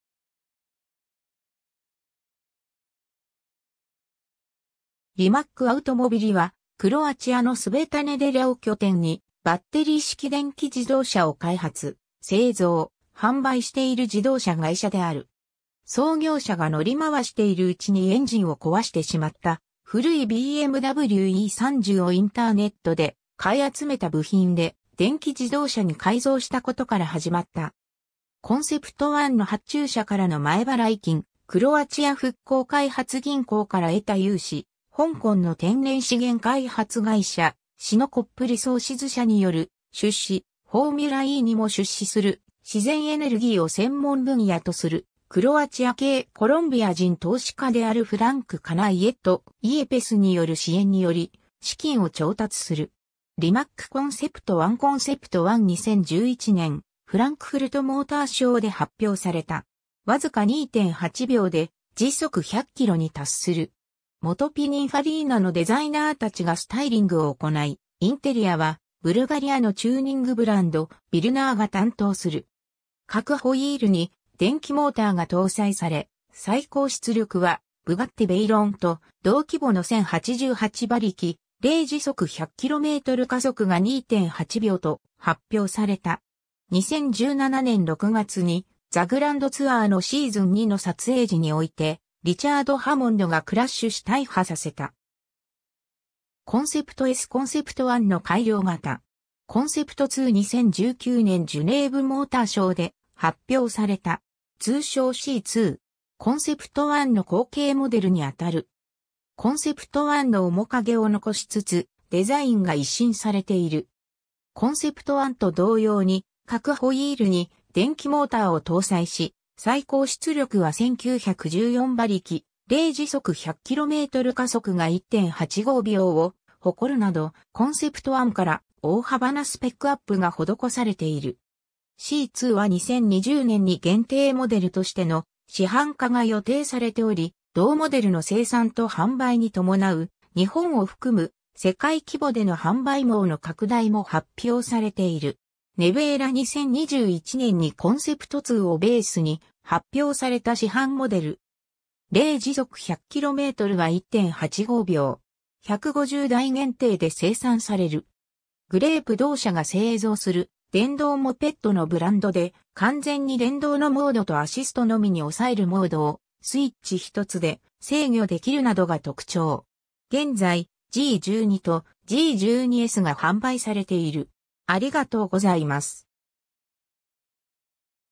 リマック・アウトモビリ
クロアチアの自動車メーカー・ブランド
リマック・アウトモビリ（英: Rimac Automobili）は、クロアチアのスベタネデリャを拠点に、バッテリー式電気自動車を開発・製造・販売している自動車会社である。
 創業者が乗り回しているうちにエンジンを壊してしまった古いBMW E30をインターネットで買い集めた部品で電気自動車に改造したことから始まった。
 コンセプト・ワンの発注者からの前払金、クロアチア復興開発銀行（HBOR）から得た融資、香港の天然資源開発会社シノコップ・リソーシズ社による出資、フォーミュラEにも出資する自然エネルギーを専門分野とするクロアチア系コロンビア人投資家であるフランク・カナイェット・イエペス(Frank Kanayet Yepes)による支援により、資金を調達する。
 リマック・コンセプト・ワン コンセプト・ワン（Concept One） 2011年フランクフルトモーターショーで発表された。
わずか2.8秒で時速100キロに達する。
元ピニンファリーナのデザイナーたちがスタイリングを行い、インテリアはブルガリアのチューニングブランド、ヴィルナーが担当する。
各ホイールに電気モーターが搭載され、最高出力はブガッティ・ヴェイロンと同規模の1088馬力、0-100km/h加速が2.8秒と発表された。
2017年6月に、「The Grand Tour」のシーズン2の撮影時において、リチャード・ハモンドがクラッシュし大破させた。
 コンセプトS（Concept S） コンセプト・ワンの改良型。
 コンセプト・ツー (Concept Two) 2019年ジュネーブモーターショーで発表された。
通称 C_Two。
コンセプト・ワンの後継モデルにあたる。
コンセプト・ワンの面影を残しつつデザインが一新されている。
コンセプト・ワンと同様に各ホイールに電気モーターを搭載し、最高出力は1914馬力、0-100km/h加速が1.85秒を誇るなどコンセプト・ワンから大幅なスペックアップが施されている。
「C_Two」は2020年に限定モデルとしての市販化が予定されており、同モデルの生産と販売に伴う、日本を含む世界規模での販売網の拡大も発表されている。
 ネヴェーラ 2021年にコンセプト・ツーをベースに発表された市販モデル。
0-100km/hは1.85秒。
150台限定で生産される。
 グレイプ 同社が製造する電動モペッドのブランドで、完全に電動のモードとアシストのみに抑えるモードを、スイッチ一つで制御できるなどが特徴。
現在、G12とG12Sが販売されている。